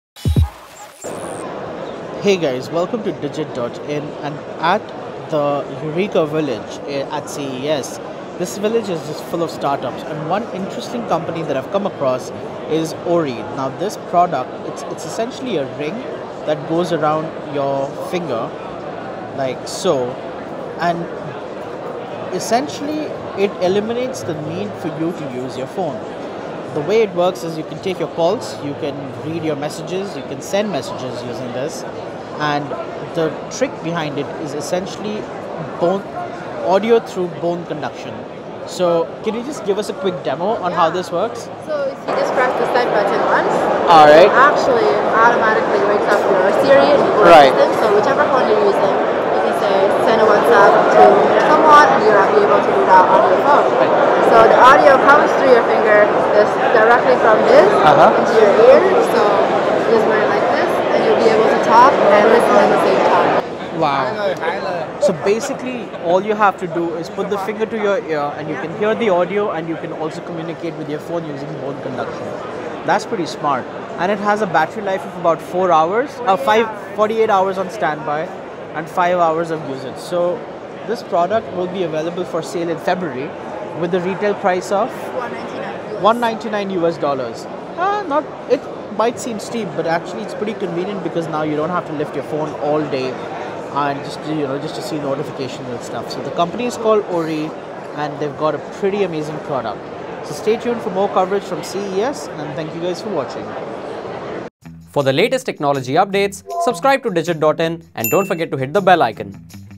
CES 2019: ORII Voice Powered Smart Ring for sending messages | Digit.in
In this video we take a look at ORII which is a voice powered smart ring for sending messages without the need of a screen. ORII was showcased at CES 2019.